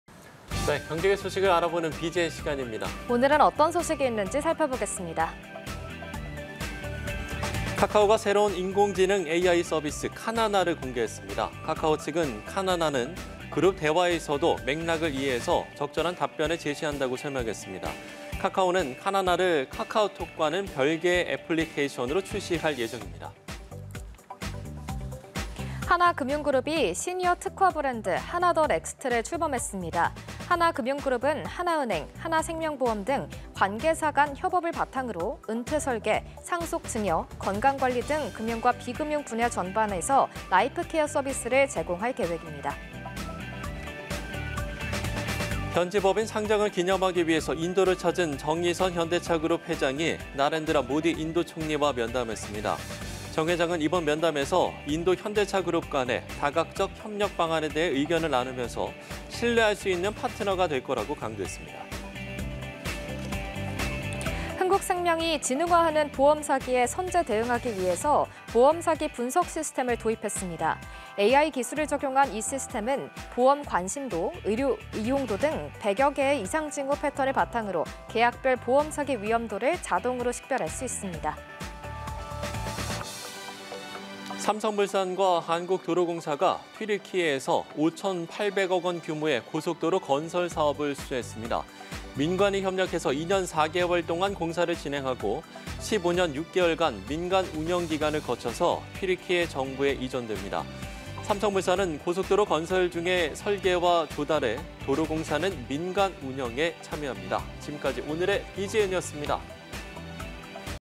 [비즈&] 카카오, 새로운 AI 서비스 '카나나' 공개 外 / 연합뉴스TV (YonhapnewsTV)
[비즈&] 카카오, 새로운 AI 서비스 '카나나' 공개 外

경제계 소식을 알아보는 비즈&(앤) 시간입니다.

오늘(22일)은 어떤 소식이 있는지 살펴보겠습니다.

▶ 카카오, 새로운 AI 서비스 '카나나' 공개

카카오가 새로운 인공지능 AI 서비스 '카나나'를 공개했습니다.

카카오 측은 "카나나는 그룹 대화에서도 맥락을 이해해 적절한 답변을 제시한다"고 설명했습니다.

카카오는 카나나를 카카오톡과는 별개의 애플리케이션으로 출시할 예정입니다.

▶ 하나금융그룹, 시니어 특화 브랜드 출범

하나금융그룹이 시니어 특화 브랜드 '하나 더 넥스트'를 출범했습니다.

하나금융그룹은 하나은행, 하나생명보험 등 관계사 간 협업을 바탕으로 은퇴설계, 상속·증여, 건강관리 등 금융과 비금융 분야 전반에서 라이프케어 서비스를 제공할 계획입니다.

▶ 정의선, 인도 총리 면담…"신뢰의 파트너 될 것"

현지법인 상장을 기념하기 위해 인도를 찾은 정의선 현대차그룹 회장이 나렌드라 모디 인도 총리와 면담했습니다.

정 회장은 이번 면담에서 인도-현대차그룹 간 다각적 협력 방안에 대해 의견을 나누면서 "신뢰할 수 있는 파트너가 될 것"이라고 강조했습니다.

▶ 흥국생명, AI 적용 보험사기 분석 시스템 도입

흥국생명이 지능화하는 보험사기에 선제 대응하기 위해 '보험사기 분석 시스템'을 도입했습니다.

AI 기술을 적용한 이 시스템은 보험 관심도, 의료 이용도 등 100여개의 이상 징후 패턴을 바탕으로 계약별 보험사기 위험도를 자동으로 식별할 수 있습니다.

▶ 삼성물산·도로공사, 튀르키예 고속도로 건설 수주

삼성물산과 한국도로공사가 튀르키예에서 5,800억 원 규모의 고속도로 건설 사업을 수주했습니다.

민관이 협력해 2년 4개월 동안 공사를 진행하며, 15년 6개월간 민간 운영 기간을 거쳐 튀르키예 정부에 이전됩니다.

삼성물산은 고속도로 건설 중 설계와 조달에, 도로공사는 민간 운영에 참여합니다.